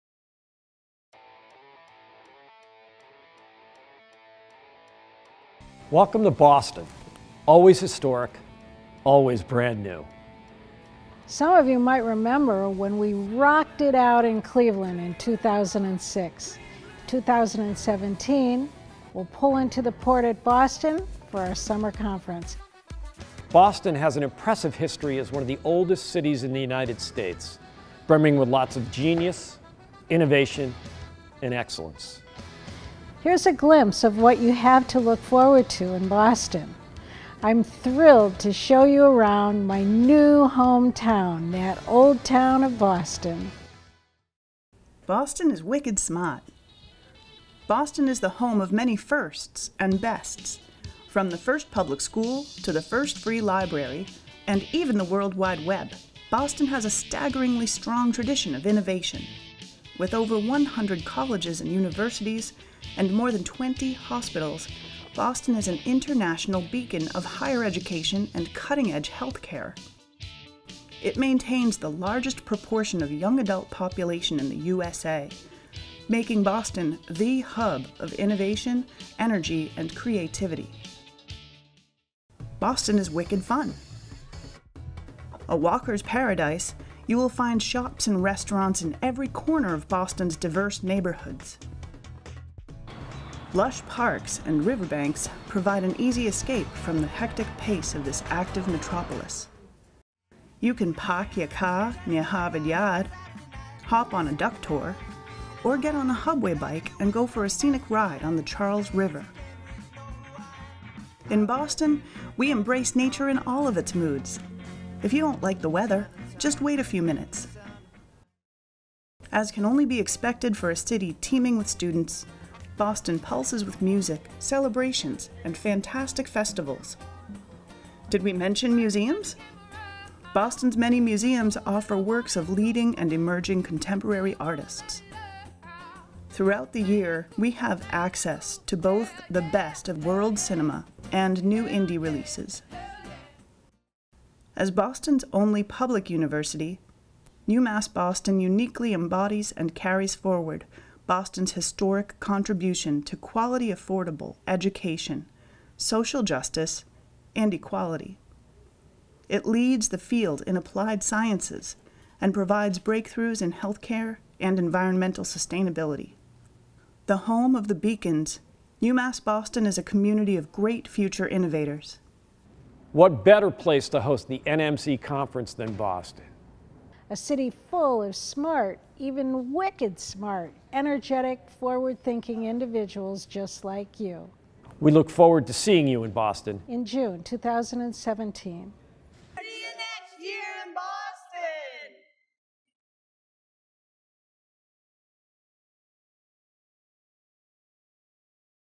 2017 NMC Summer Conference Invitation
UMass Boston is the host of the 2017 New Media Consortium Summer Conference.